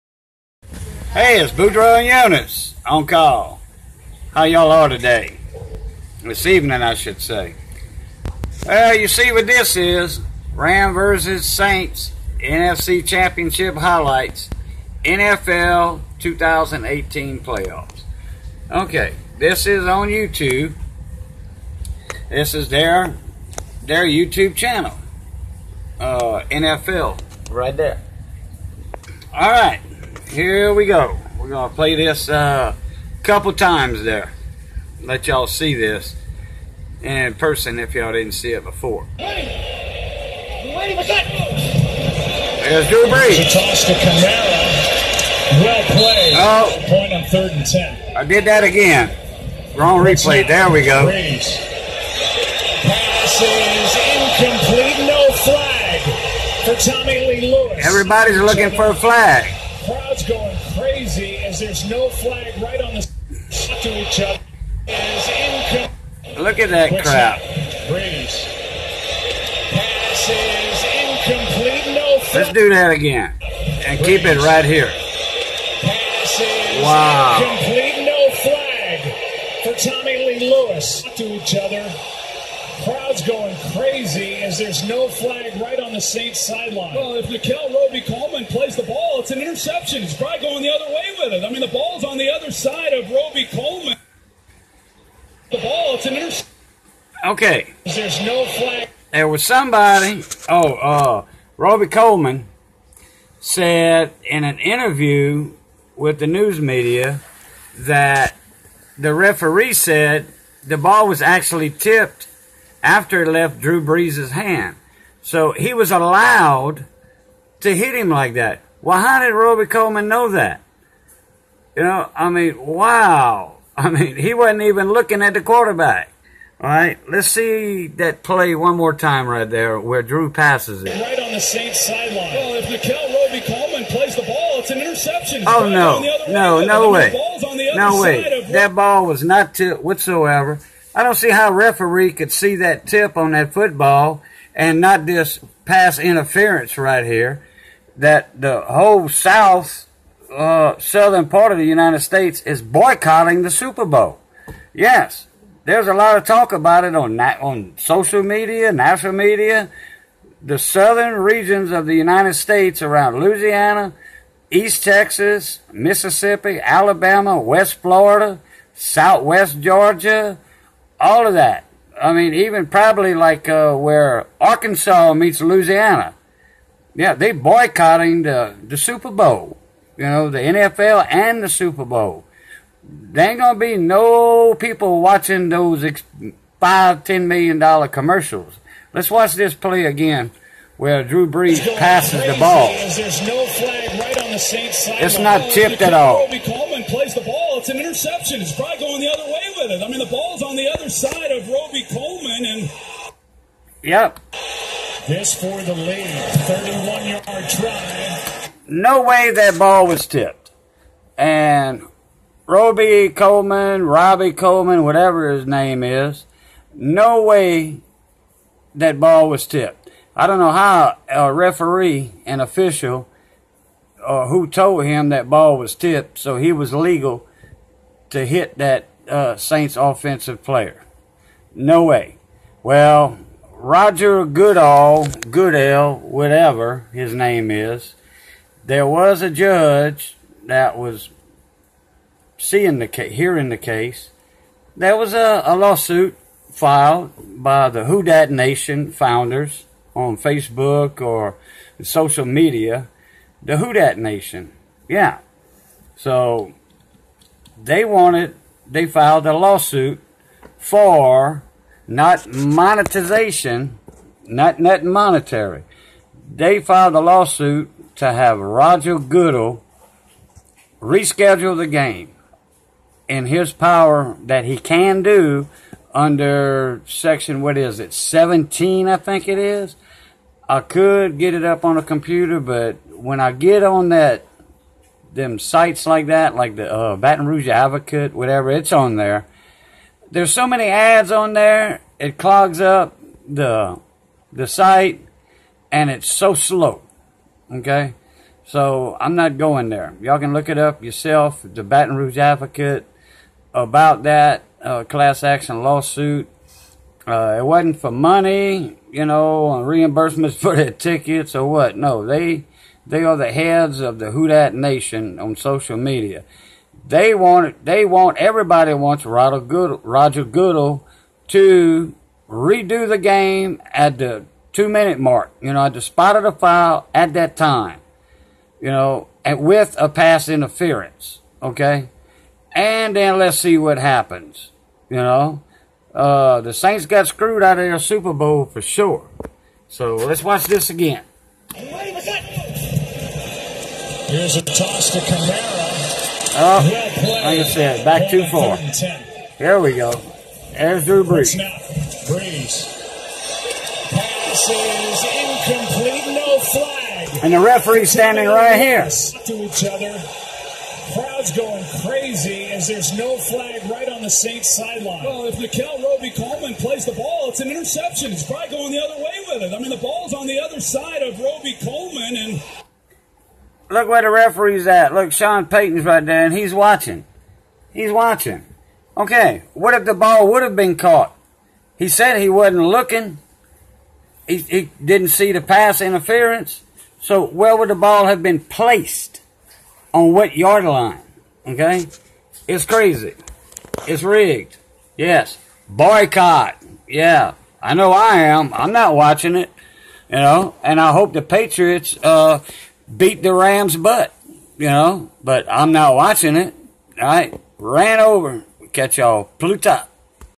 Super Bowl is Boycotted in the Southern USA !!!!!!
There is so much controversy about this NO CALL play in this game.

It's going down in history as the most destructive thing to happen in NFL history.

Will there be a new rule in the 2019 season for a NO CALL for a coach to see a replay ?

There was so much I didn't talk about in the video. Something needs to be done about this.